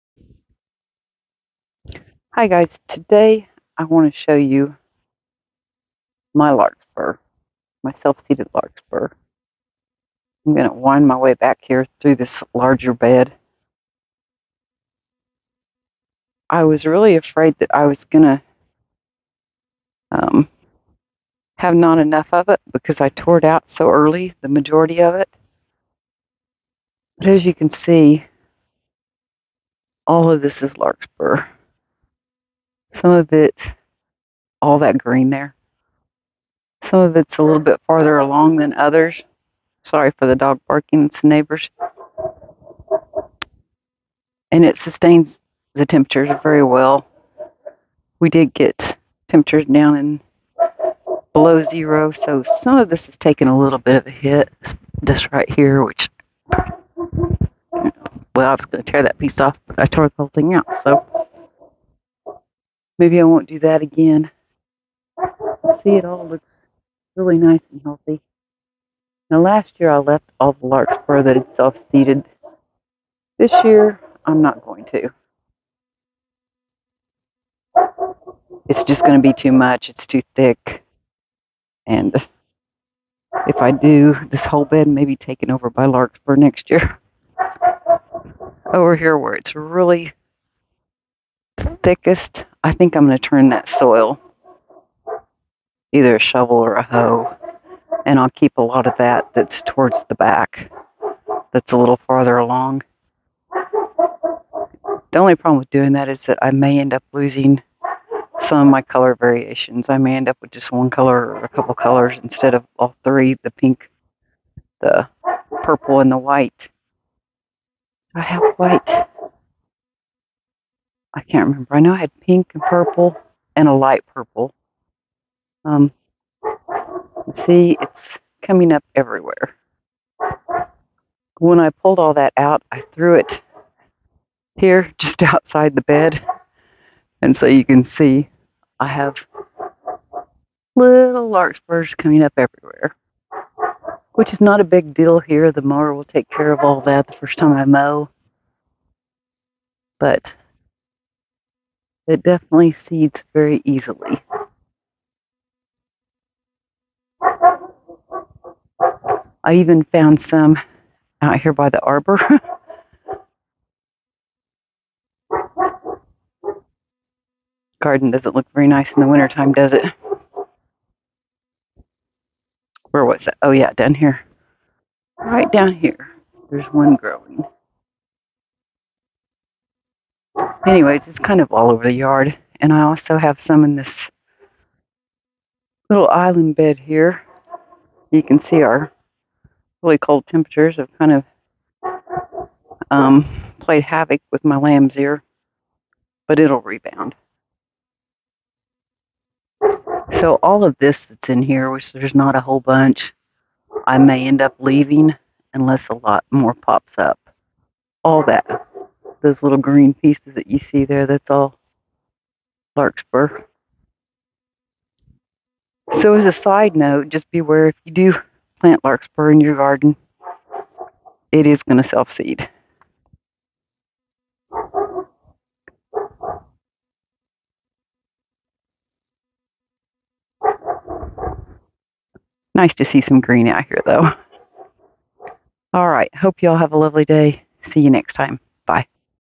Larkspur has over seeded itself!
I think I have too much self-seeded larkspur.  Thanks to everyone who watches my videos and leaves a comment, I appreciate it.
Until next time, Kay.